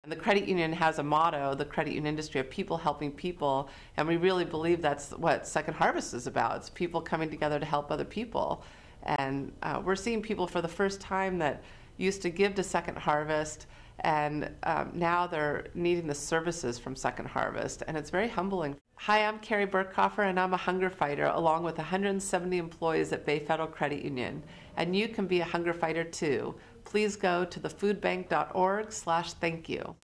Carrie Birkhofer Hunger Fighter Second Harvest Food Bank
Carrie Birkhofer leads Bay Federal Community Credit Union, its 170 employees and over 55,000 members to fighting hunger in Santa Cruz County. Bay Federal has been a presenting sponsor of the Holiday Food Drive for a number of years and they are proud to be a part of the hunger fighter army.